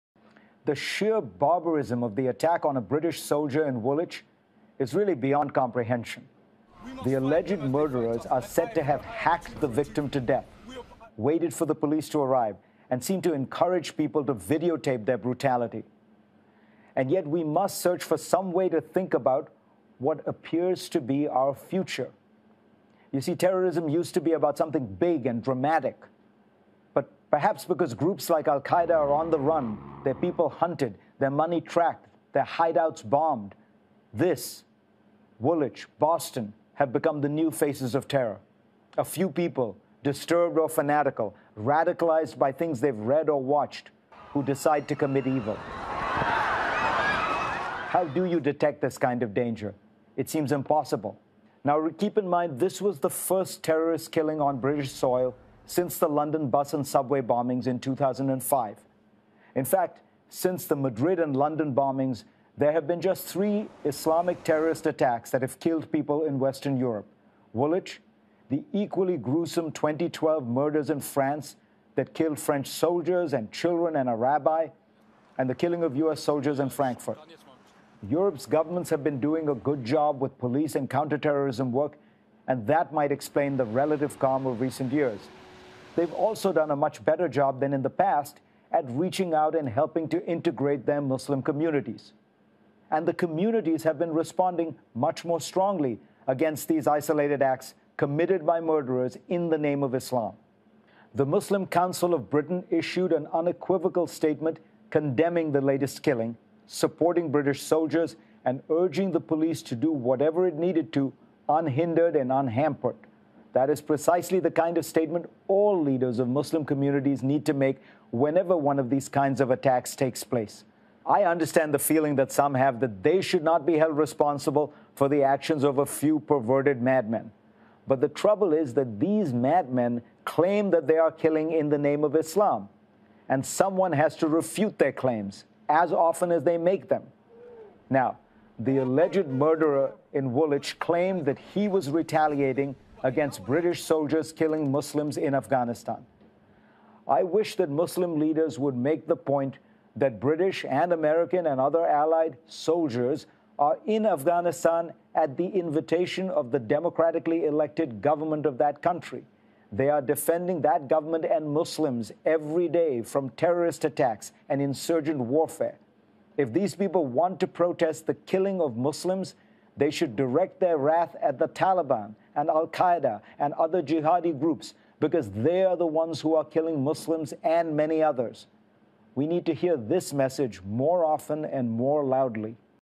Did Woolwich Begin to Change How The Media Sees Radical Islam?
Fareed Zakaria gives his take on the terrorist attack in Woolwich, and how to approach terrorism. Is this the new normal or just a momentary hiatus from the suicidal insanity of Kamikaze Multiculturalism?